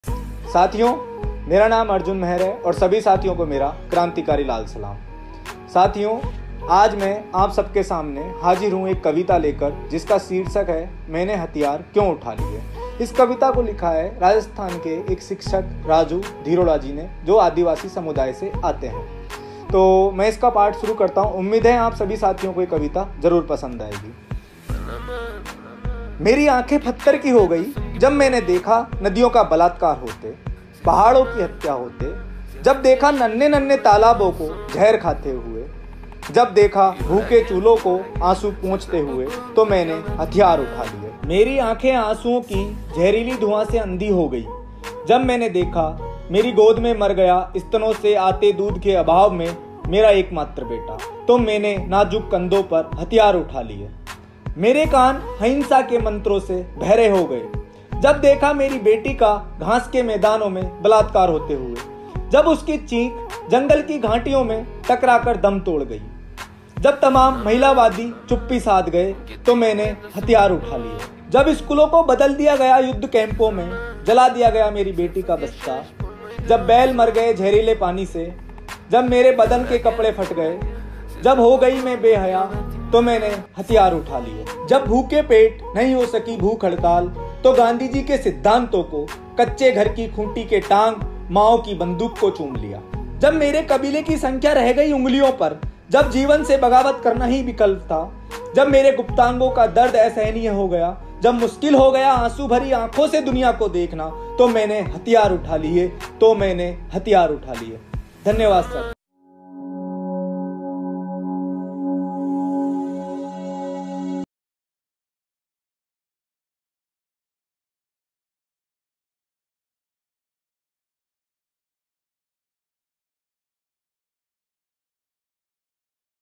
मैंने हथियार क्यों उठायें | Maine Hathiyar Kyon Uthaye | Arjun Mehar | Raju Dheeroda | Hindi Poetry
मैंने हथियार क्यों उठायें | Maine Hathiyar Kyon Uthaye | ArjunMehar | Raju Dheeroda

मेरी आँखें पत्थर की हो गई,
जब मैंने देखा,
नदियों का बलात्कार होते,
पहाड़ो की हत्या होते,
जब देखा नन्हे नन्हे तालाबो को,
जहर खाते हुए,
जब देखा भूखे चूल्हों को,
आंसू पोंछते हुए,
तो मैंने हथियार उठा लिए।
©️राजू धीरोडा